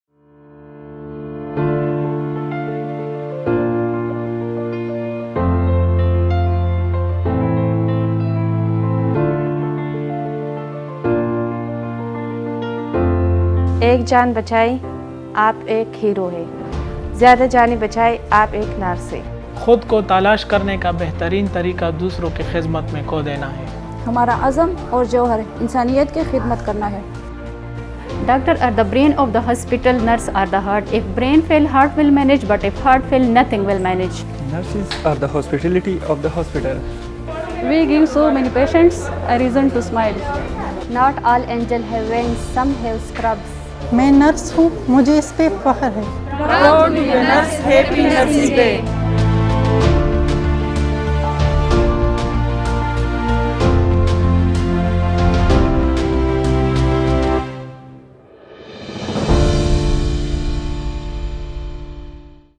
International Nursing Day | 12th May 2023 | Khyber Teaching Hospital
Nurses are a unique kind. They have this insatiable need to care for others, which is both their greatest strength and fatal flaw!

Not all superheroes wear capes. Here's taking the opportunity to applaud the superheroes of the KTH Nursing Department for tirelessly working towards our health and safety.

Our nurses are the backbone of the healthcare industry, ensuring that patients receive the best possible care and treatment. The theme for the 2022 resources is "Nurses: A Voice to Lead - Invest in Nursing and respect rights to secure global health"

As we approach International Nursing Day 2023 Khyber Teaching Hospital nursing staff shared different messages highlighting the significance of InternationalNursesDay.